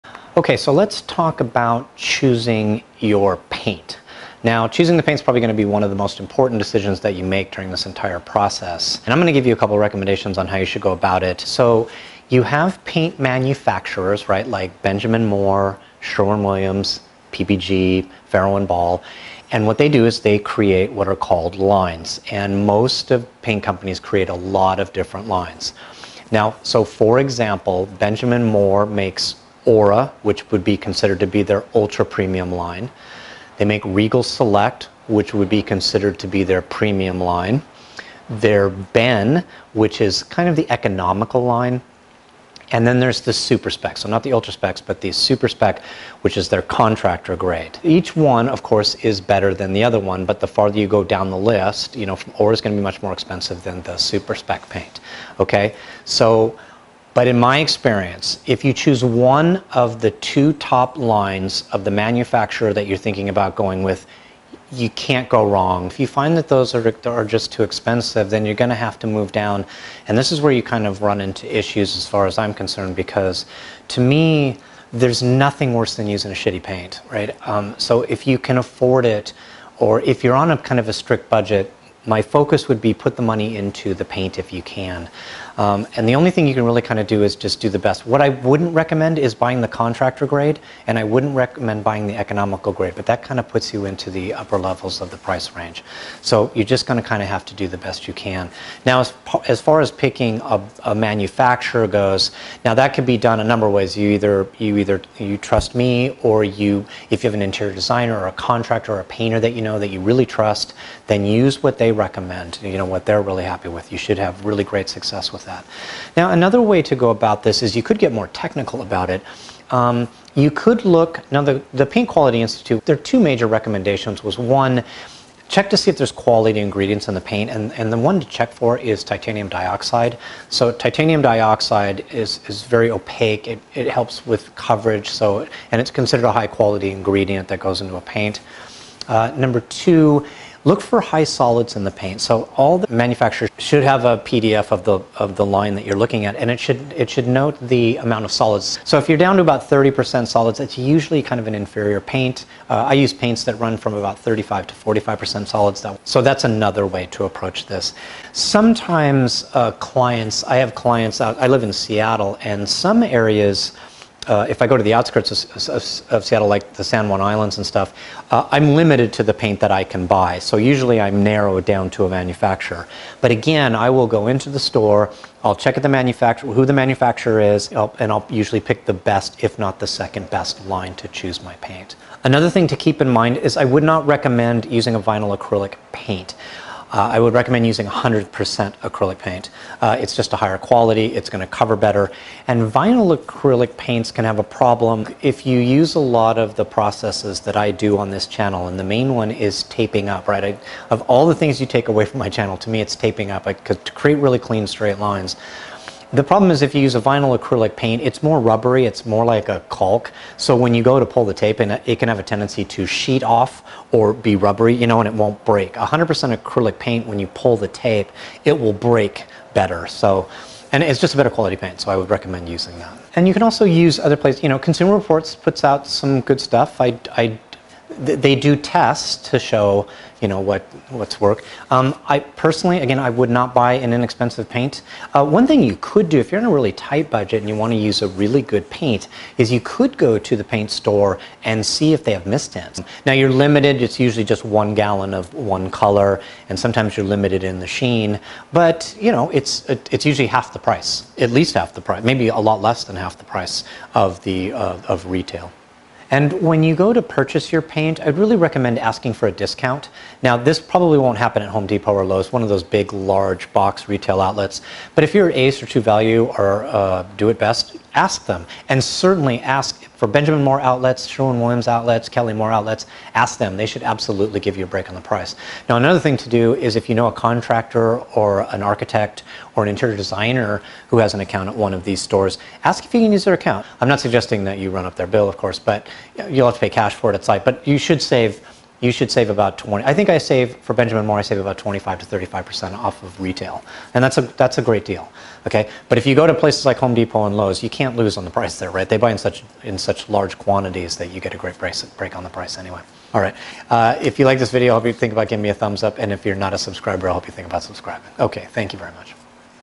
How To Pick The Right Paint
I discuss some tips on how to choose your paint.  I cover choosing manufacturers, lines, what to look for, and some thoughts on purchasing the paint.